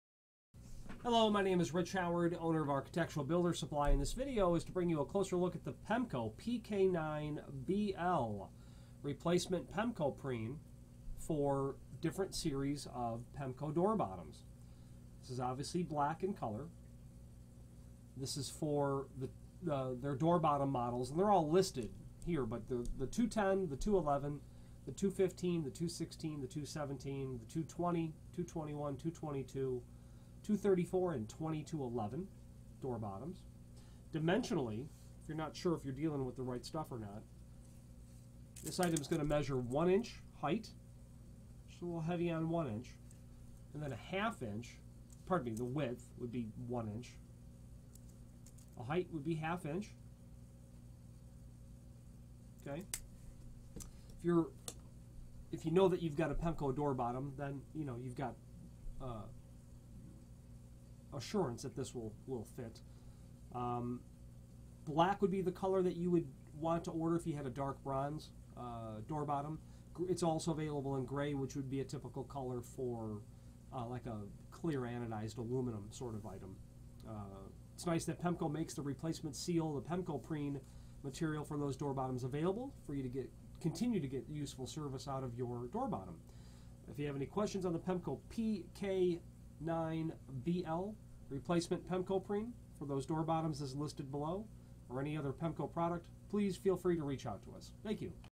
Pemko PK9BL Replacement PemkoPrene - Black
This video is to bring you a closer look at the Pemko PK9BL Replacement PemkoPrene - Black.
This is for the many different series of the Pemko door bottoms (210, 211, 215, 216, 217, 220, 221, 222, 234, 2211).
Dimensionally this has a width of 1", the height is 1/2".
Black would be the color you'd want if you had a dark bronze door bottom.
This is also available in gray, this would be a typical color for a clear anodized aluminum sort of item.
It is nice that Pemko offers this replacement prene so that you can continue to get the best possible use out of your door bottom.